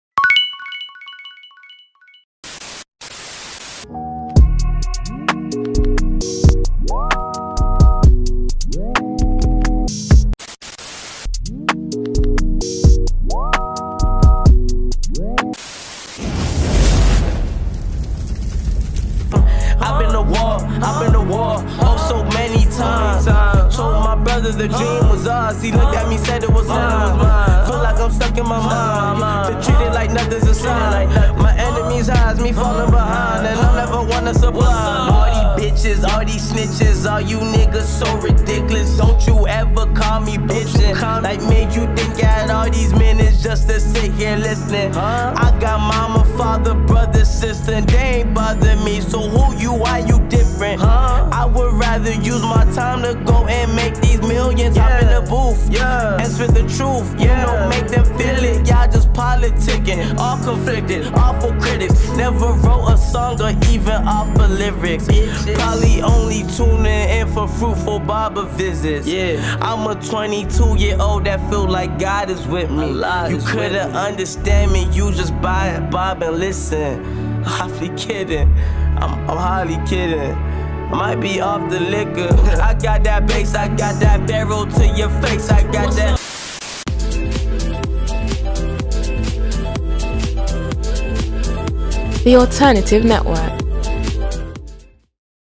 Rappers Rapper [S1.EP4]: Ka$h
On this episode of Rappers Rapper we invited Ka$h to spit some bars.